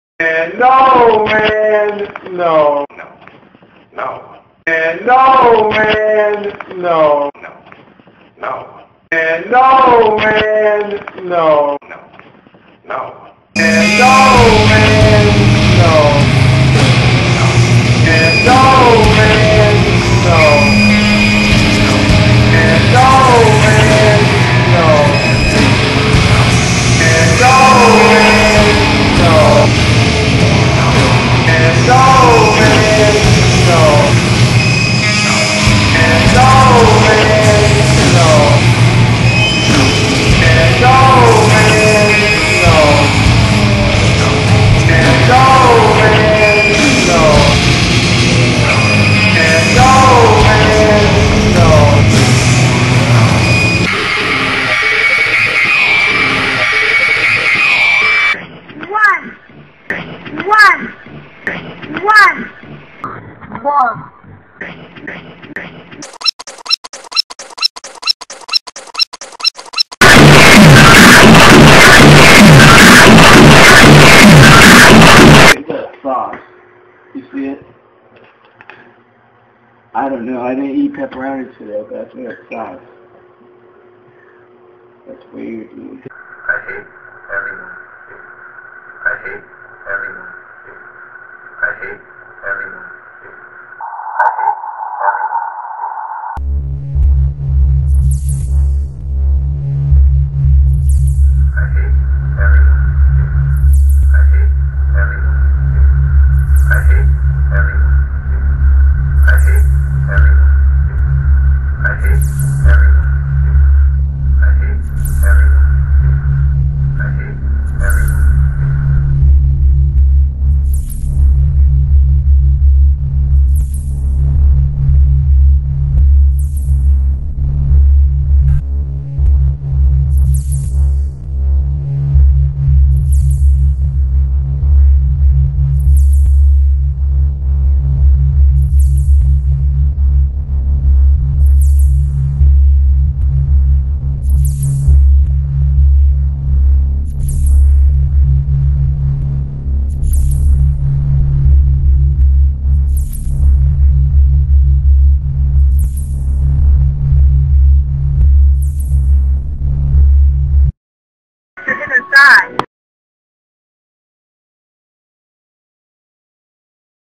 NONONONONONONONONONO
Hellomynameischad and the Joker get down to some Khanate, When all of the sudden we realize Diego Saucedo is sleeping, they try to do work outs to wake him up, but it just makes Hellomynameischad throw up pizza Sauce, he passes out and begins to have a dream about how much he hates everyone, and dancing to black metal while wearing some corpsepaint, he gets lost in his dream thinking of Diego Saucedo sleeping and how insane it made him, remembers a time whilst watching Zack Galifianakis, when suddenly he is abruptly woken up to his work out routine again, to find that the Joker had now left.